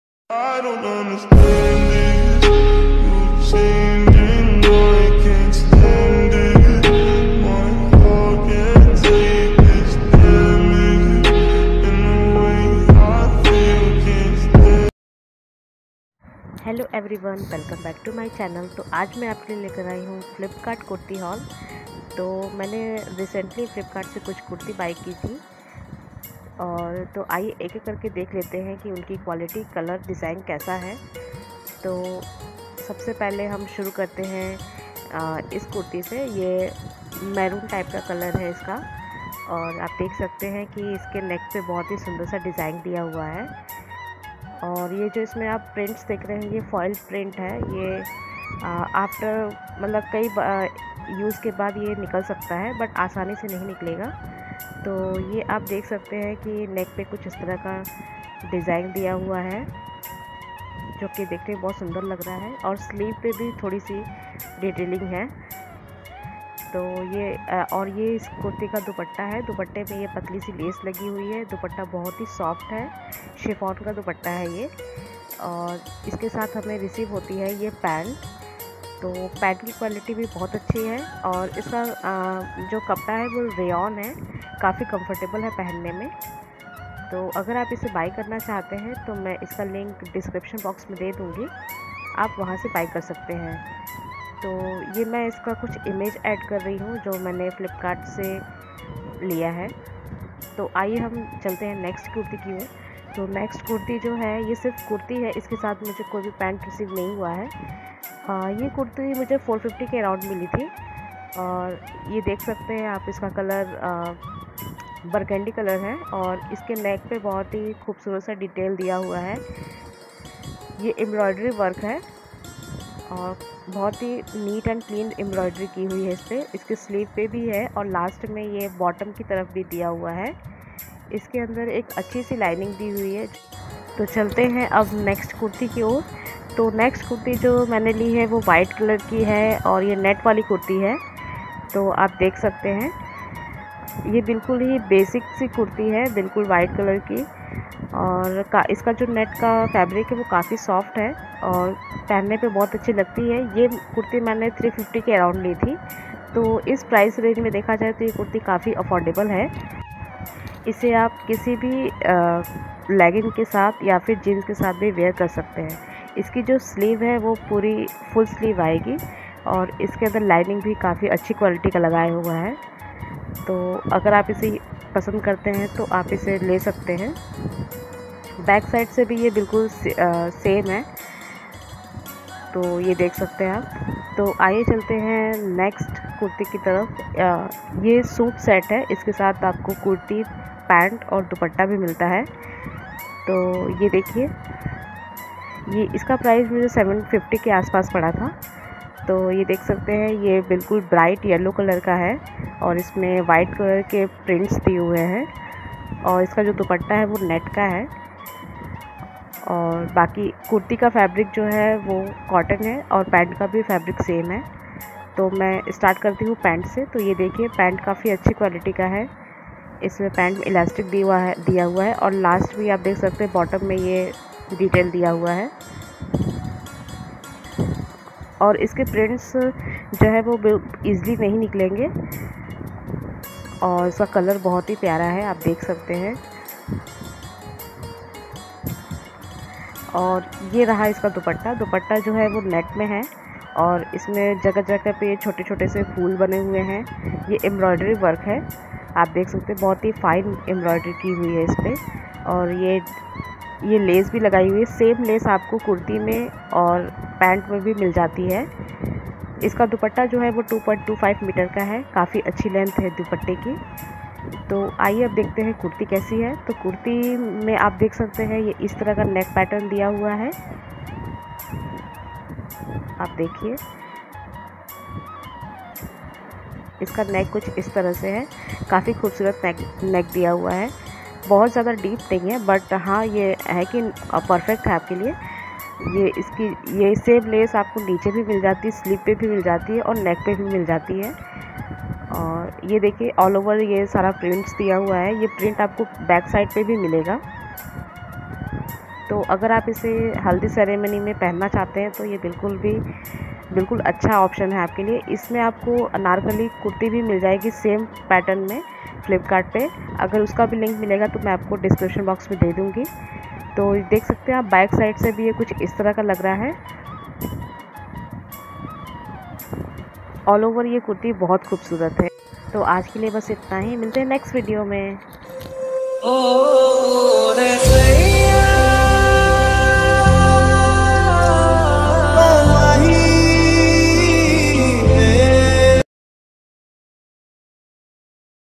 Flipkart Summer Kurti Haul|| Affordable Summer Kurti Haul
Today I am sharing with you another amazing recipe..

Thankyou soo much..

Product link:
1. Wine kurta pant set

2.Wine kurti

3.White net kurti

4. yellow straight kurti pant set

yellow anarkali kurti pant set

Thankyou friends😀😀